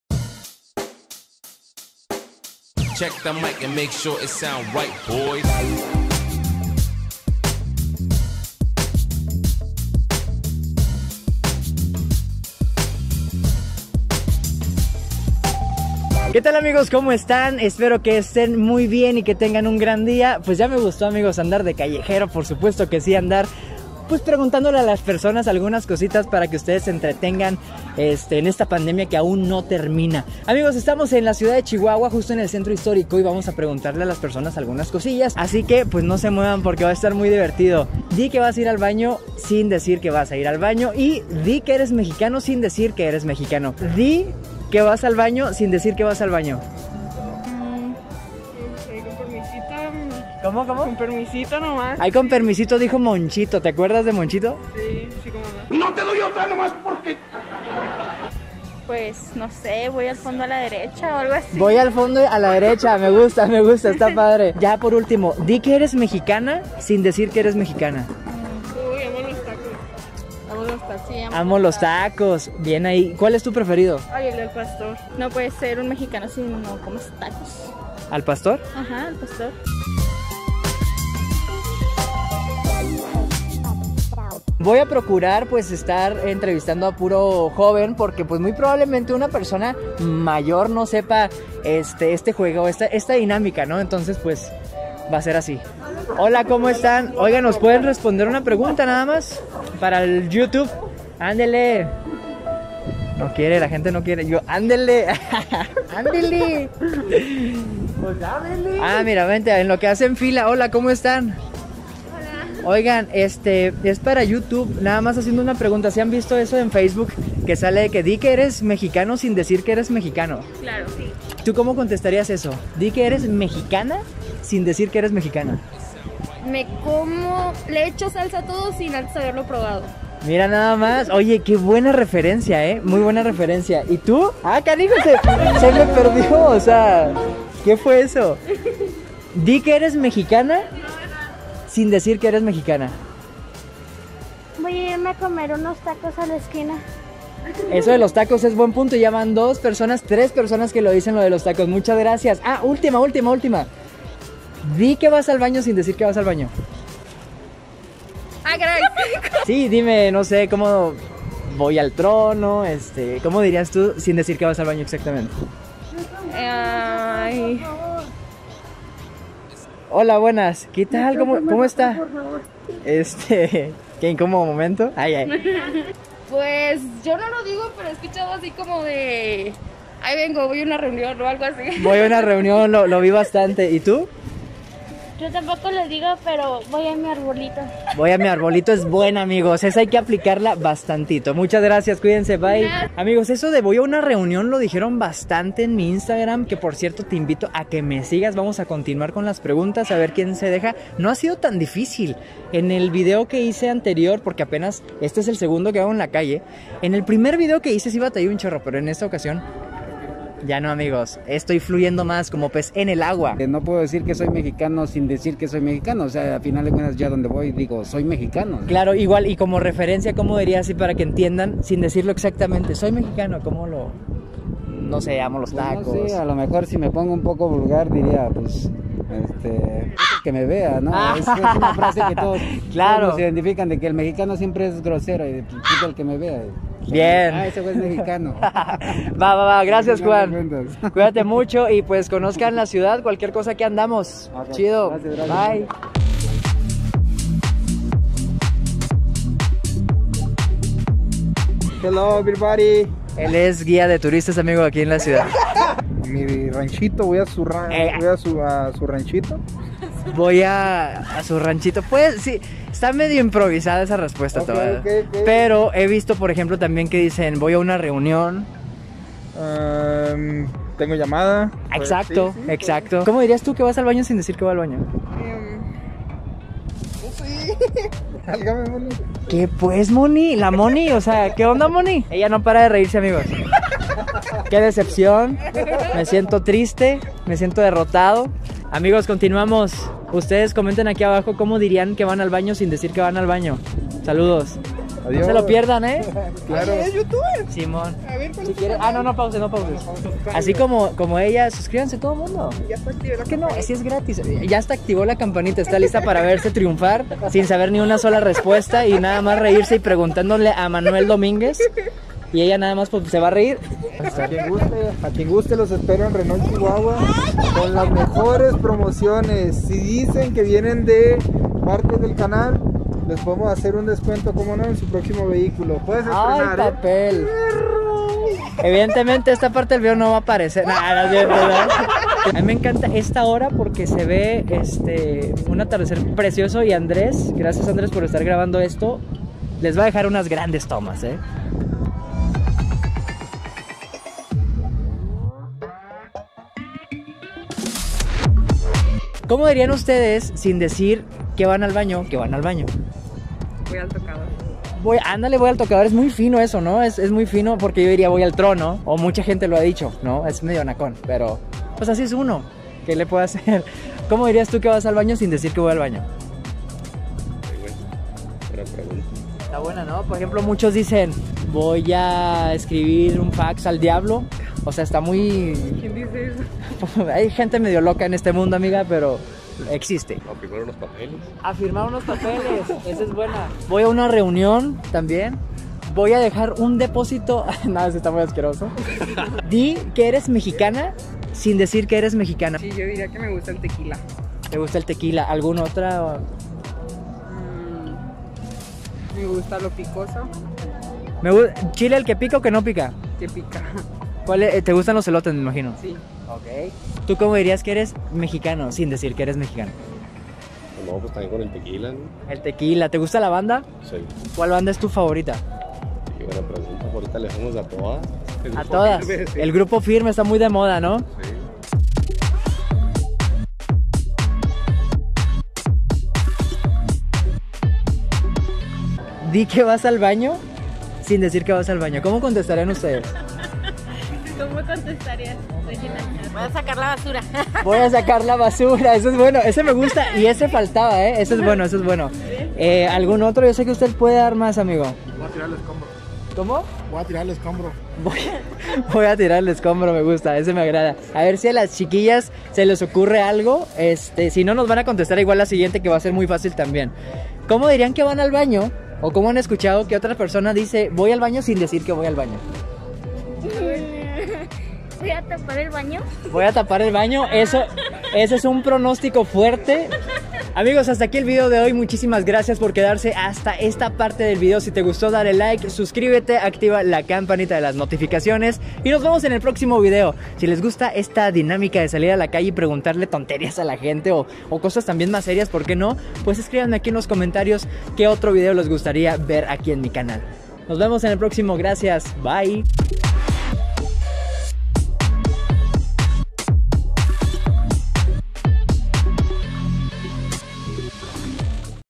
DI QUE ERES MEXICANO Y QUE VAS AL BAÑO SIN DECIRLO | RETO xD
Hola amigos! Esta ocasión salí a entrevistar algunas personas en la ciudad de Chihuahua